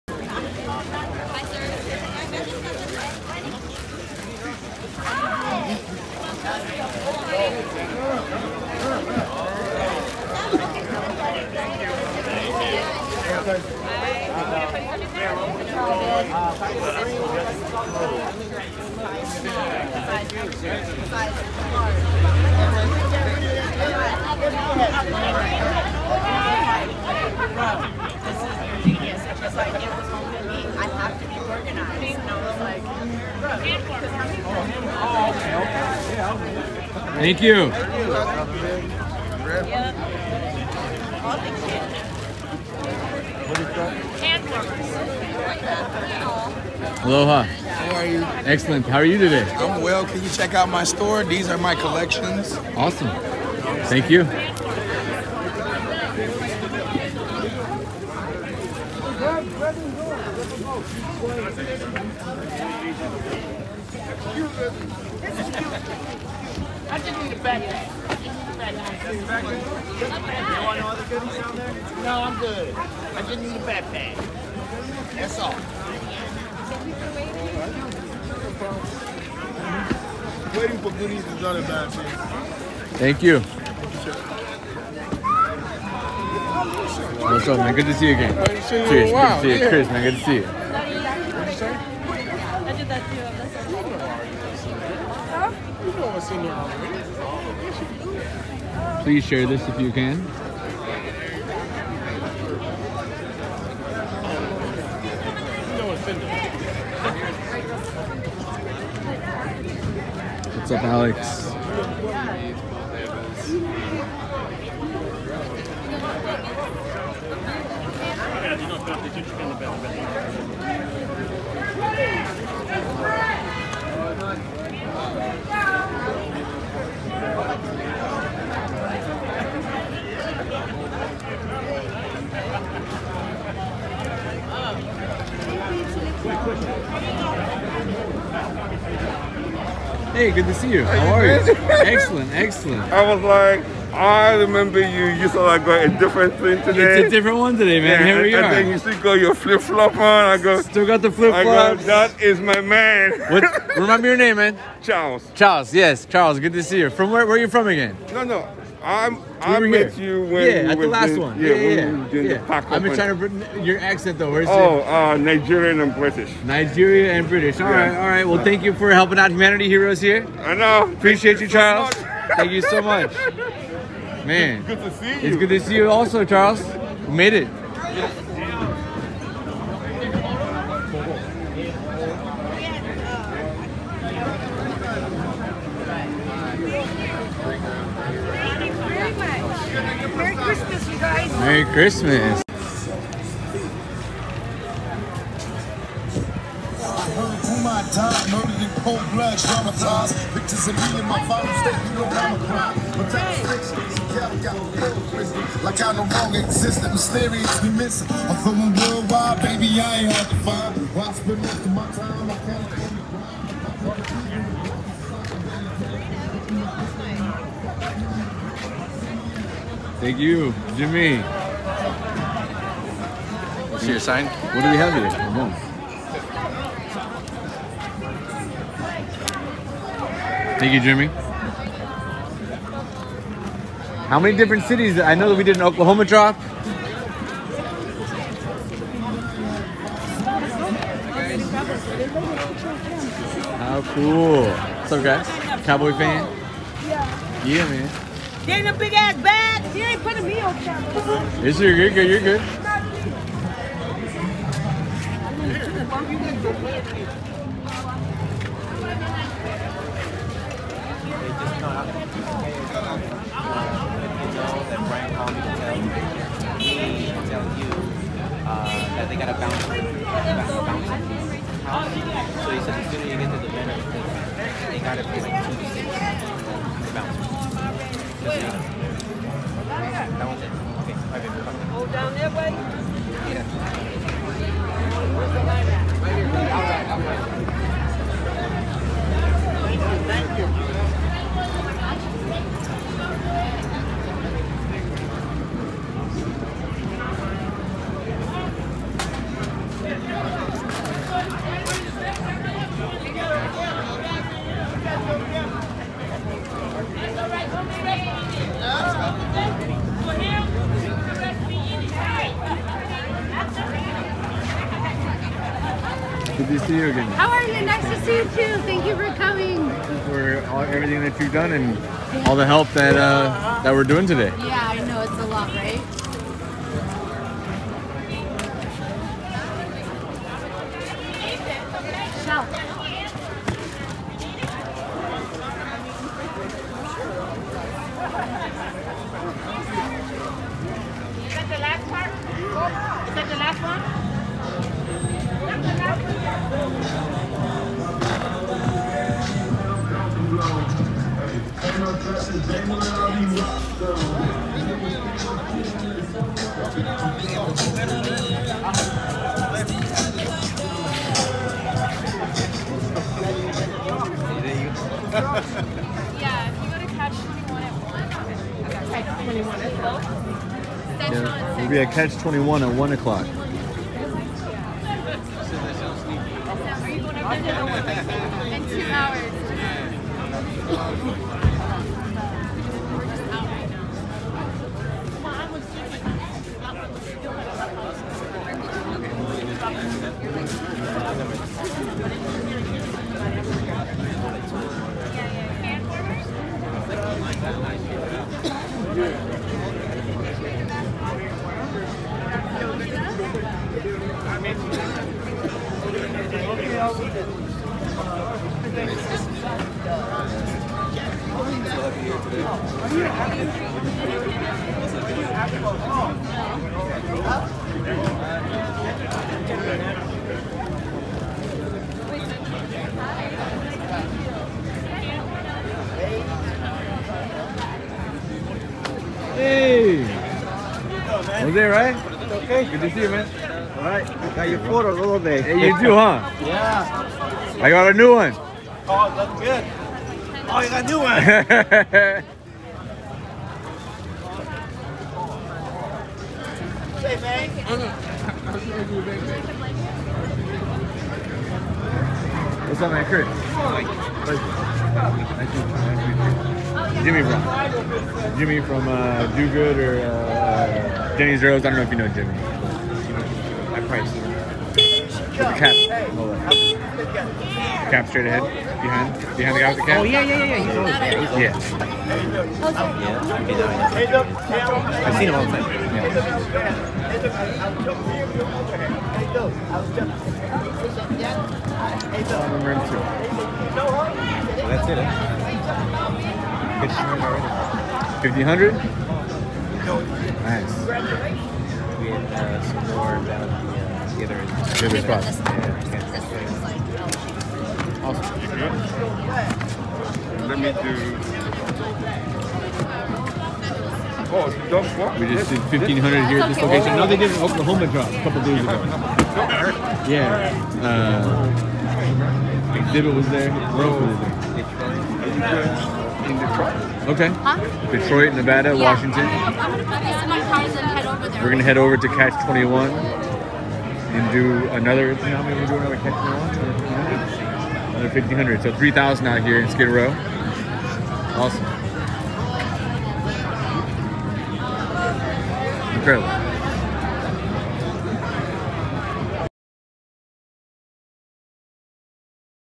Giving 1,500 backpacks to homeless on Skid Row, 10,000 bags in total: Connecting With The Community
Humanity Heroes along with Do Good International and Advanced Nutrients helping when and where it’s needed most.